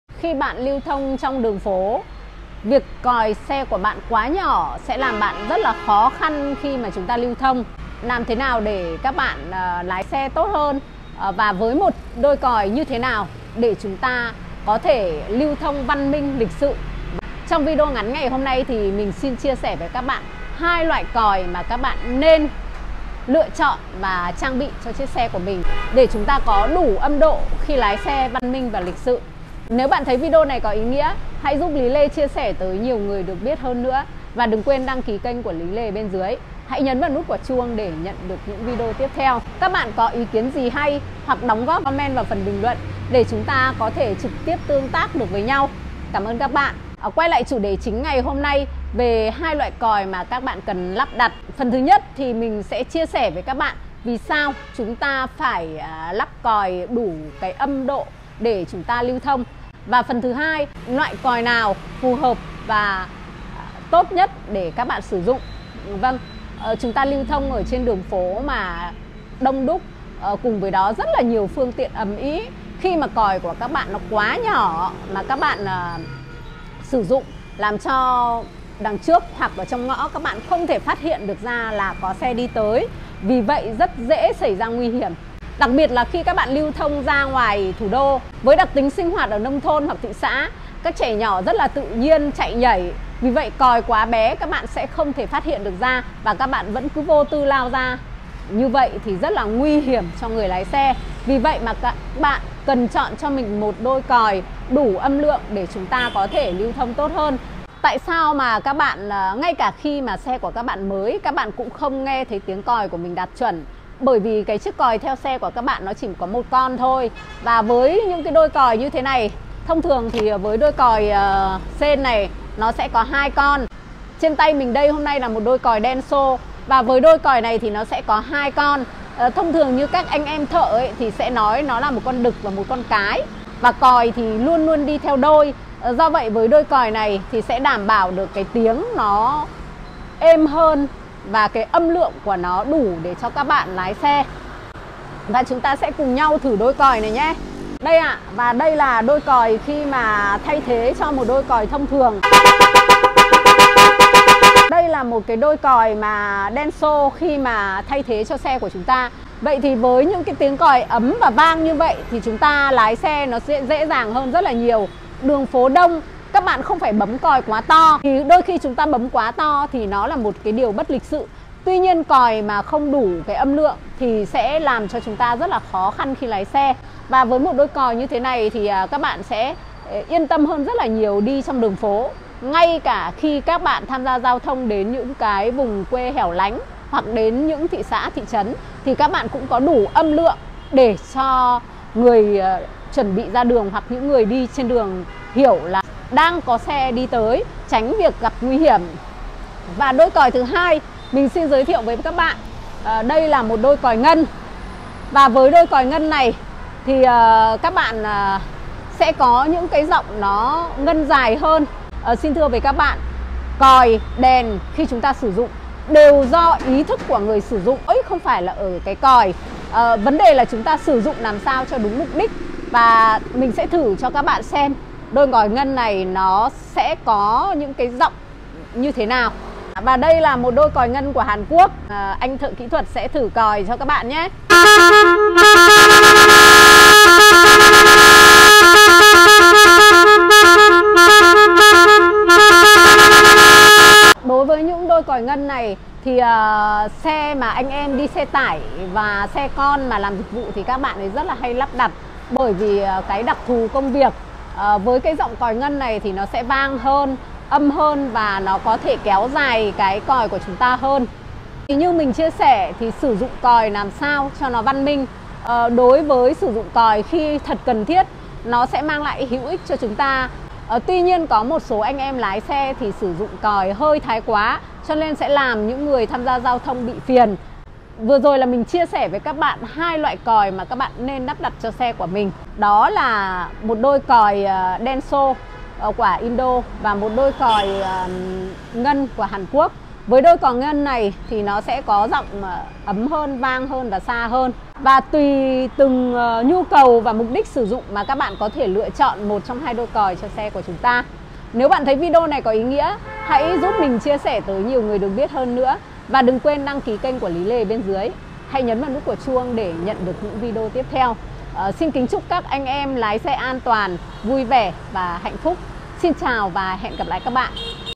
Hướng dẫn chọn CÒI XE cho xế yêu I Lý Lê
Hướng dẫn chọn còi xe cho xế yêu, còi xe là thiết bị rất quan trọng và cần thiết. Còi giúp bạn người tham gia giao thông biết xe bạn đang di chuyển và xin đường. Tuy nhiên còi quá bé không đảm bảo âm lượng để người tham gia biết, còi to quá sẽ làm người tham gia giao thông giật mình. Vậy lựa chọn còi cũng là một nghệ thuật. Bạn hãy lựa chọn cho xế yêu 1 đôi còi tốt và phù hợp nhé. Còi Denso, còi Ngân…và rất rất nhiều loại

➤Kết nối với LÝ LÊ